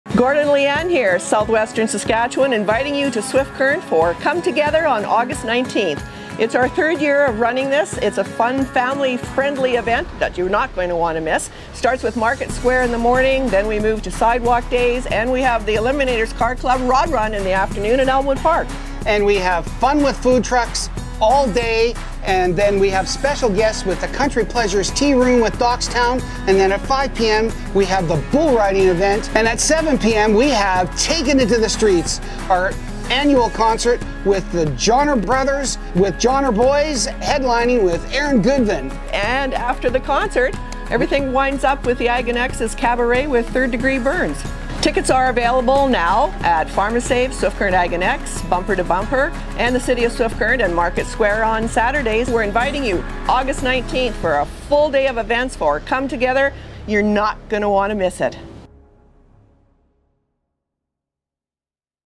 Swift Current's Come Together Event on August 19 2023
Learn all about what's on the agenda for Swift Current's Come Together Event on August 19, 2023! There's something for everyone!